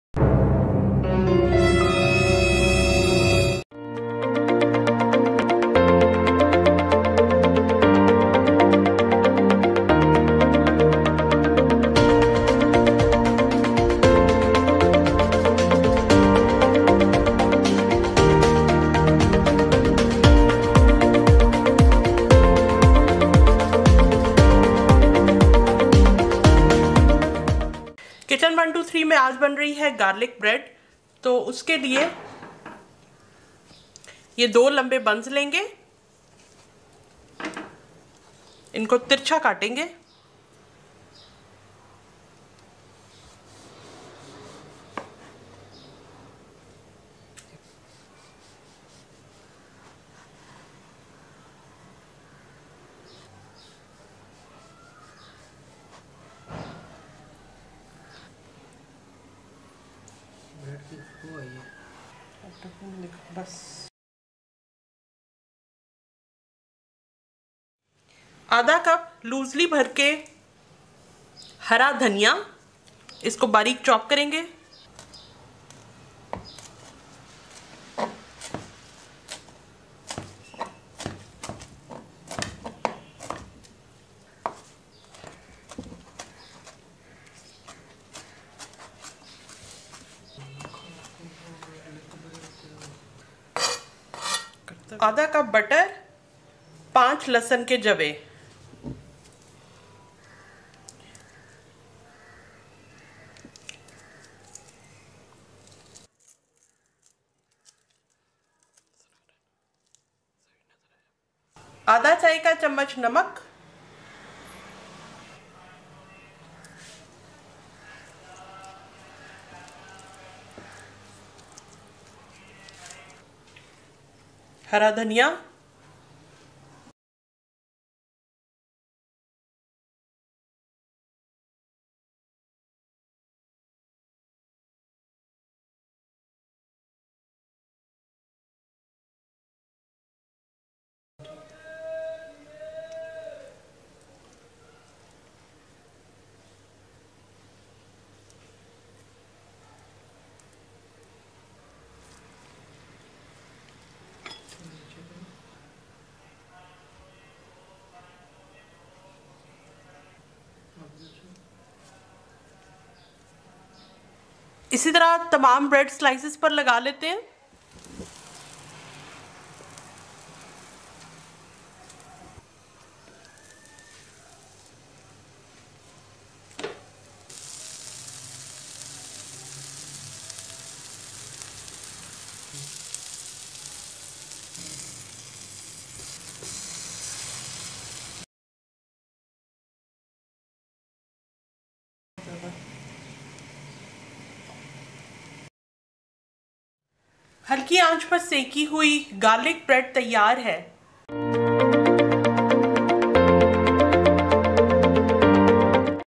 Garlic Bread | Garlic Bread at home | Crispy Garlic Bread
How to make crispy and tasty Garlic Bread at home?

INGREDIENTS

Long buns / French bread)...2
Butter-Half cup
Garlic- 5 Cloves
Salt -Half teaspoon
Green coriander)...  Half cup

RECIPE
1.Cut long buns into slices.
Keep them covered .
2.Cut green coriander leaves.
3.In butter, add freshly chopped garlic cloves.
4. Add salt and chopped coriander leaves.
5.Fold all ingredients well in butter .
6.Apply this mixture generously on both sides of bread slices.
7.On low flame, toast both sides   till become crispy.
8.Serve immediately as dinner starter or
with fried chicken.